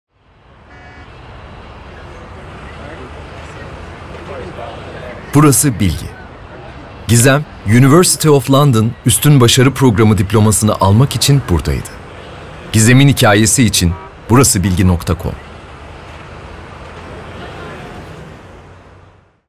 BurasıBiLGi / Gizem - Londra 2015
Gizem, University of London Üstün Başarı Programı diplomasını almak için buradaydı.
Londra 2005